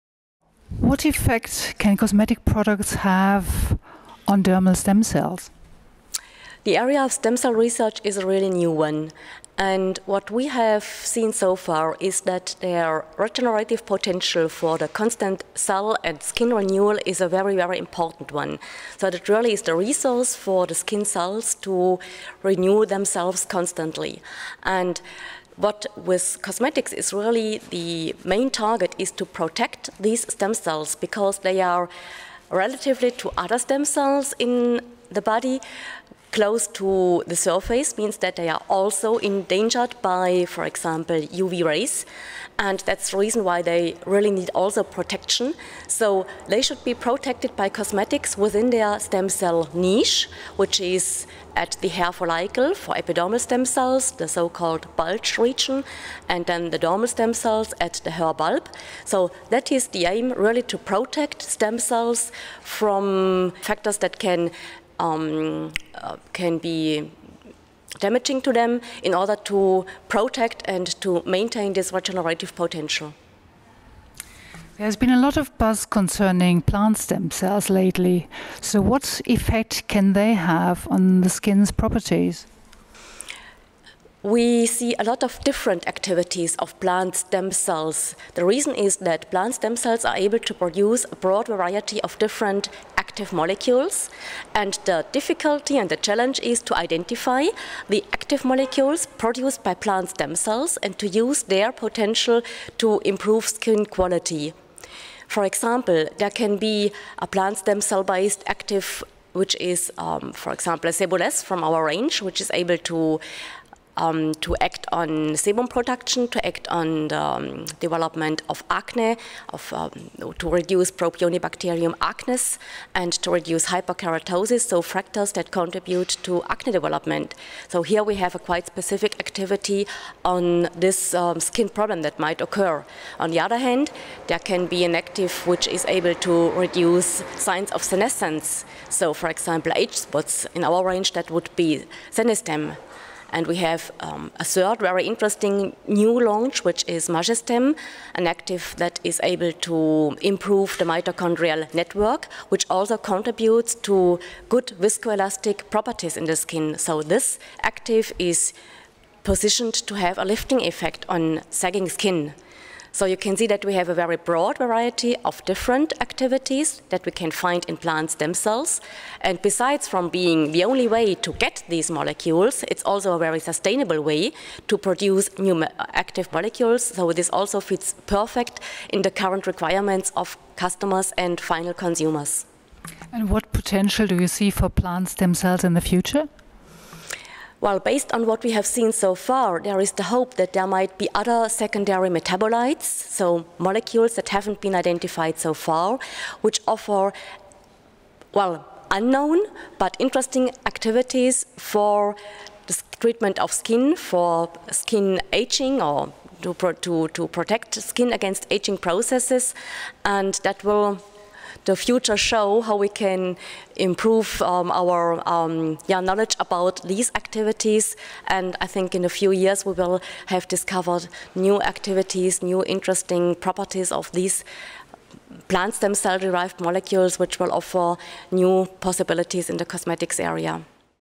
Angela Kleiner (Sederma): Stem cells and cosmetic actives
Angela Kleiner, Marketing manager at Croda and Sederma GmbH, talks about what effect cosmetics products can have on dermal stem cells. Kleiner also points out what effect plant stem cells can have on the skin's properties. She explains that one of the major challenges when developing actives from stem cells is to find the active molecules in the plant itself and to identify their potential on skin biology.